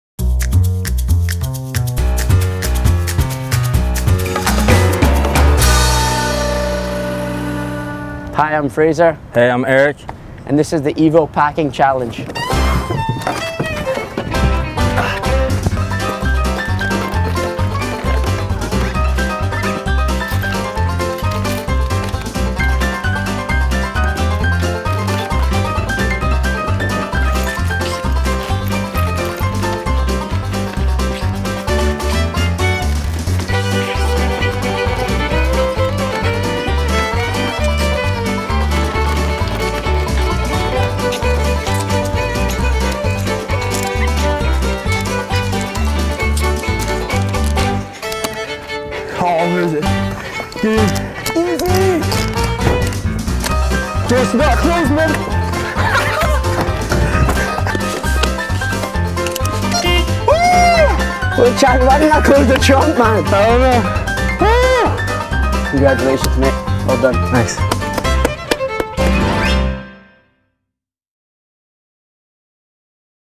BCAA | Whitecaps FC Pack an Evo Challenge
We teamed up with Vancouver Whitecaps FC to see who can pack an Evo Car Share the quickest!

Who will rise to the top in the ‘Packing an Evo Challenge’?! Erik Hurtado or Fraser Aird of the Vancouver Whitecaps FC?